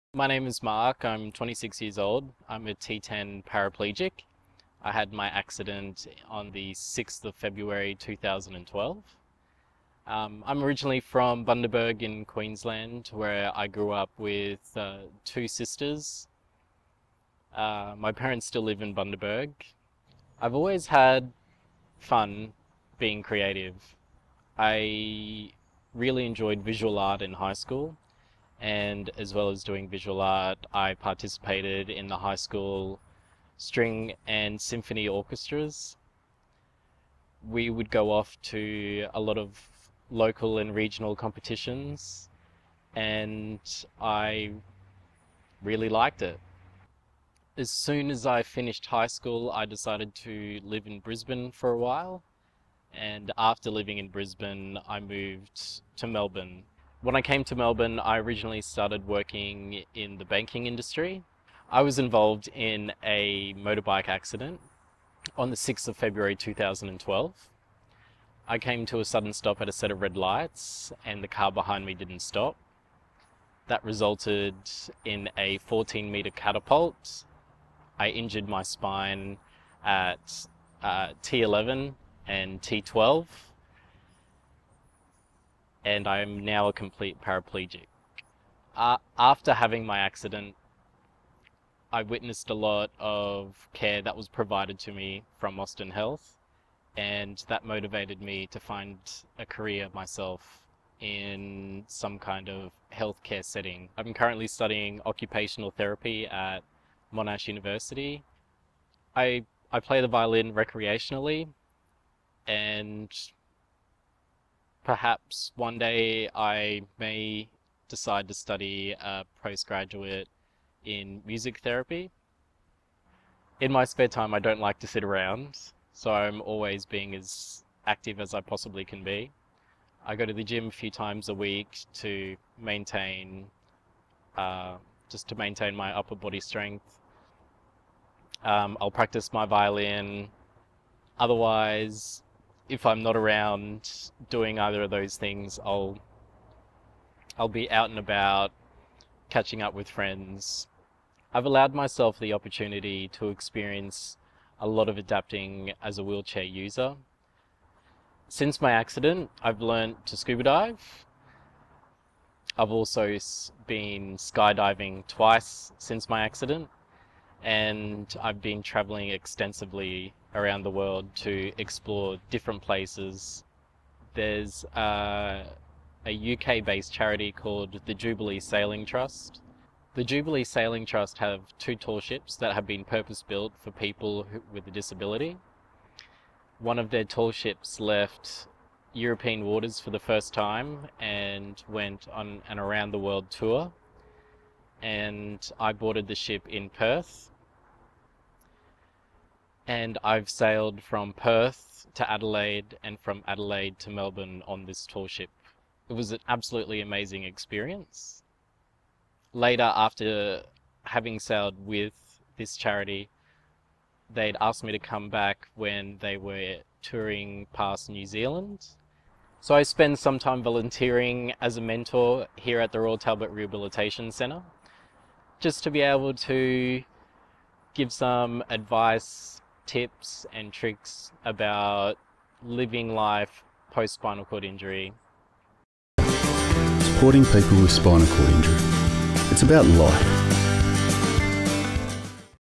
Mark Hanson
Spire peer support volunteer Mark Hanson shares some of his story about living a full life with spinal cord injury